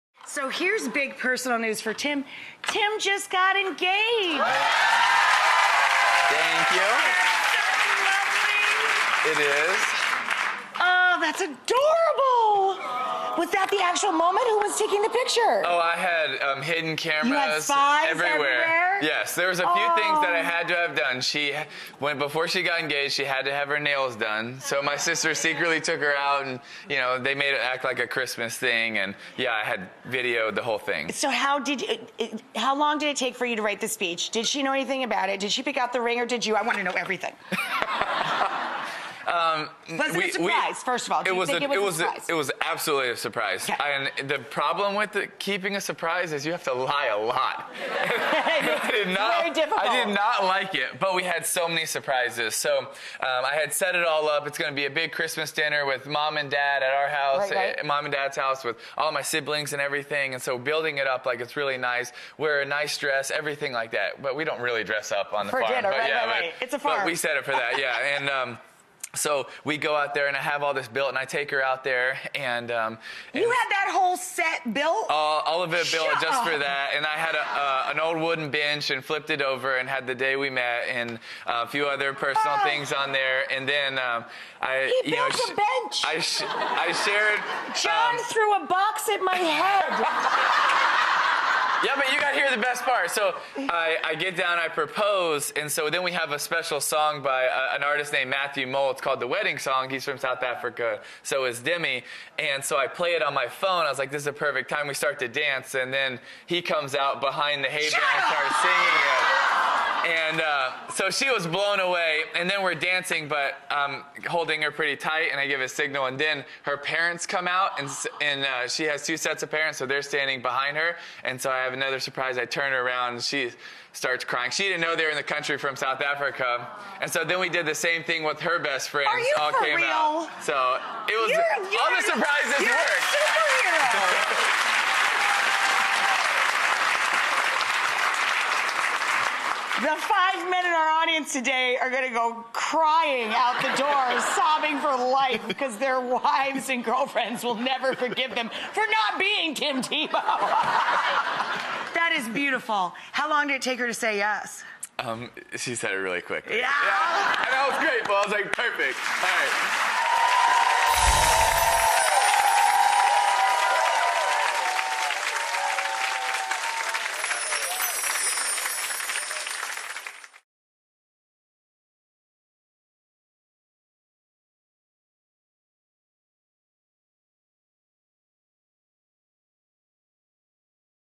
Tim Tebow On His Surprise Fairytale Proposal To Girlfriend Demi-Leigh Nel-Peters
The athlete, author and filmmaker dishes on his recent engagement to Miss Universe 2017 Demi-Leigh Nel-Peters.